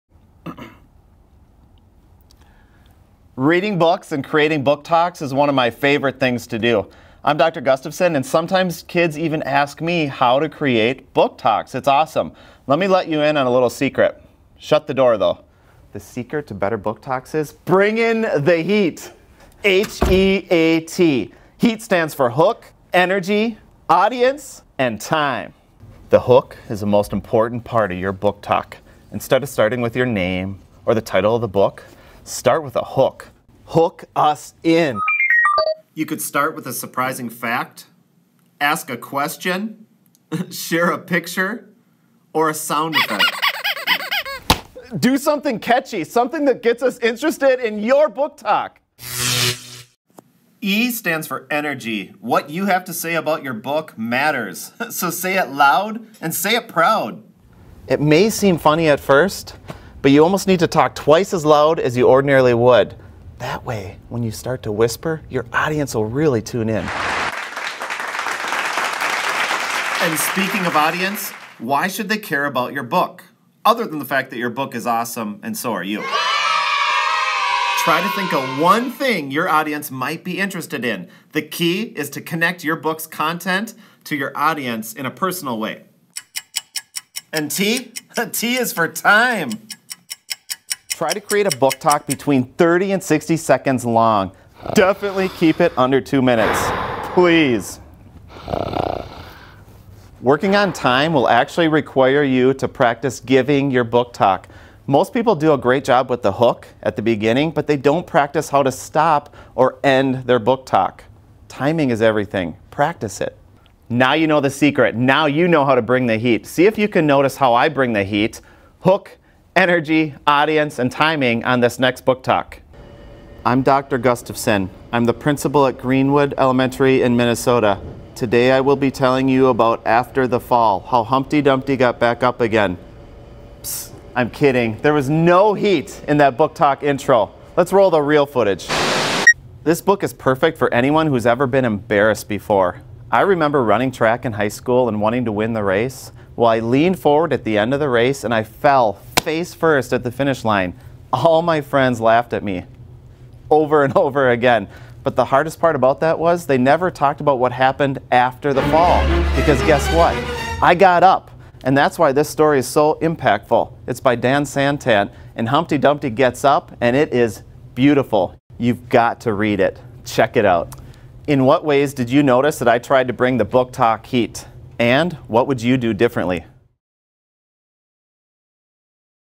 Build Better Booktalks
The secret to building better booktalks is bringing the HEAT!  This short (3 minute) video will demonstrate how to help students create compelling book recommendations using video and a memorable acronym.

Special thanks to Jeffrey Demars for helping to produce this student tutorial.